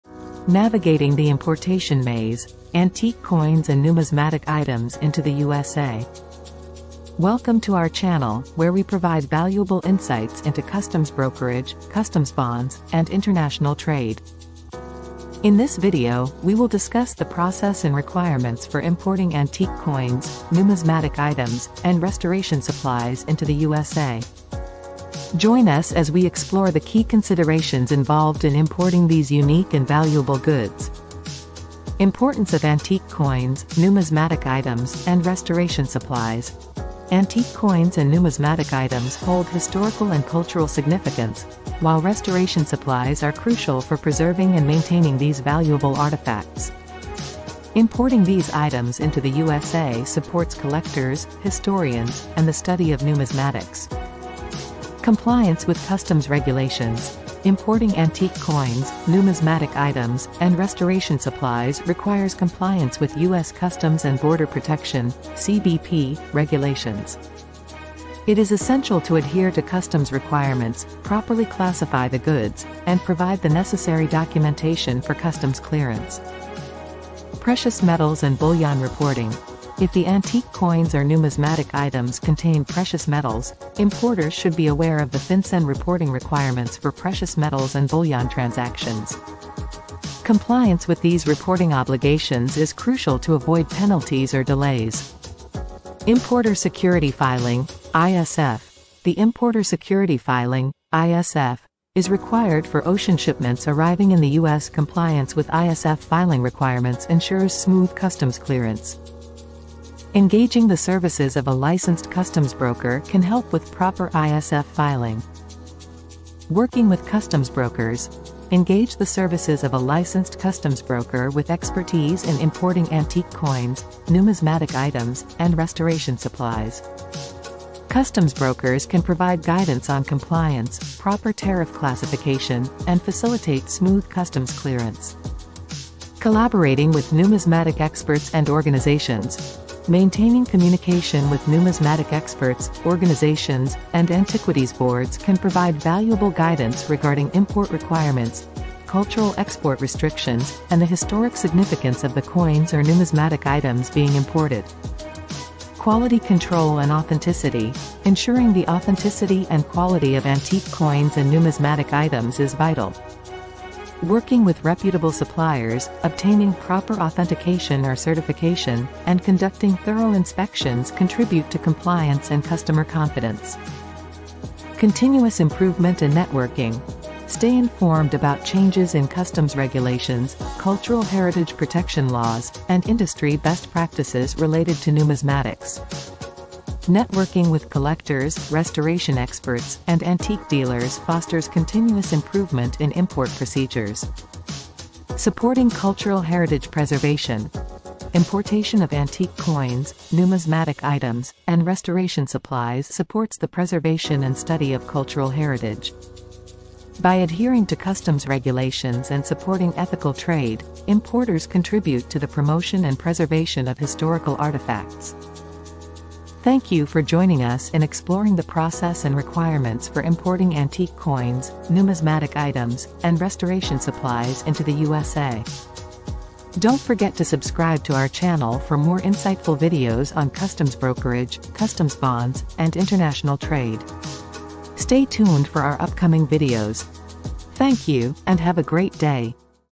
Navigating the Importation Maze: Antique Coins and Numismatic Items into the USA
ISF Customs Broker

In this video, we'll discuss the process of importing antique coins, numismatic items, and restoration supplies into the USA. We'll explore the specific regulations and requirements set forth by Customs and Border Protection (CBP) for these types of imports, focusing on aspects such as customs clearance, Importer Security Filing (ISF), and Customs Bonds. By understanding the intricacies of importing these goods, you'll be better equipped to navigate the importation process successfully.